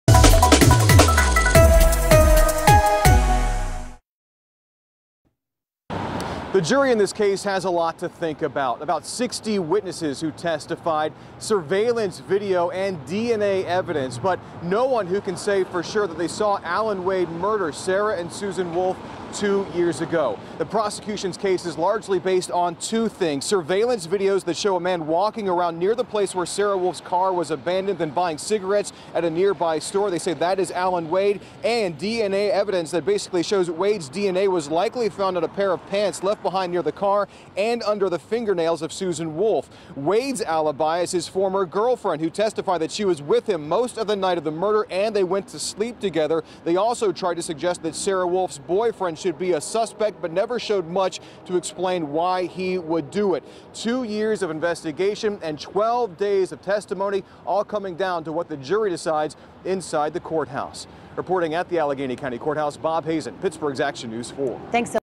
Closing arguments begin in trial of Allen Wade
After going through days of testimony and evidence, closing argument began in the trial of Allen Wade, accused of murdering two sisters in 2014.